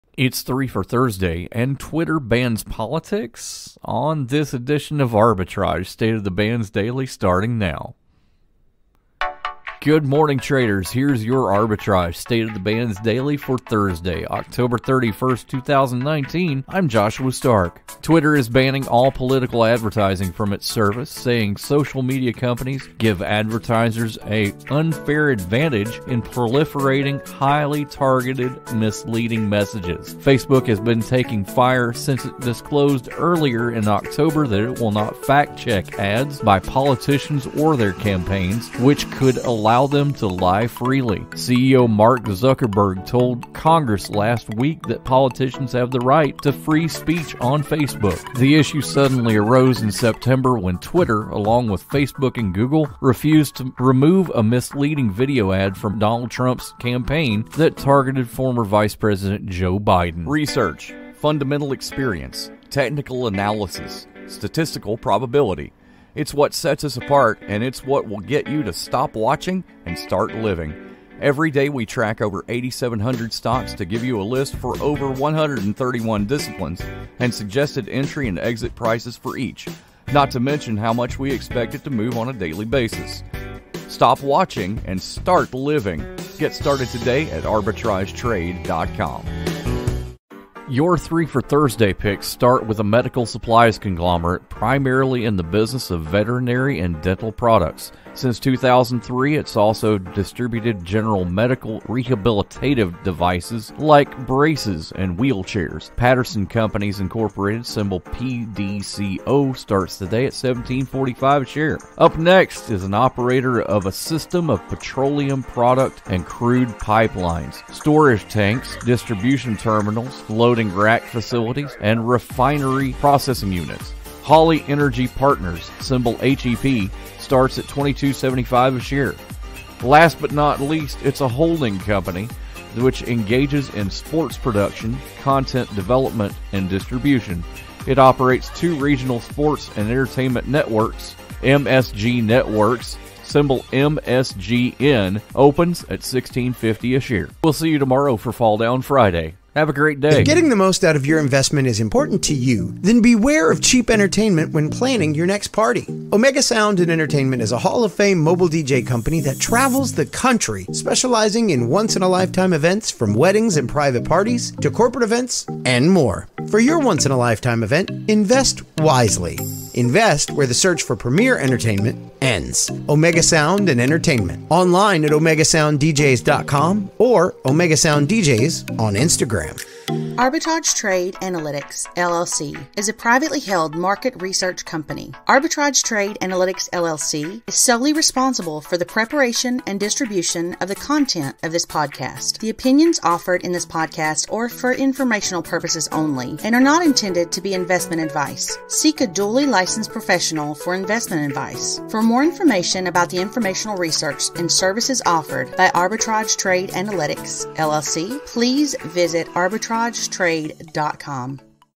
October 31, 2019
Its Three for Thursday and Twitter bans politics? on this edition of Arbitrage State of the Bands Daily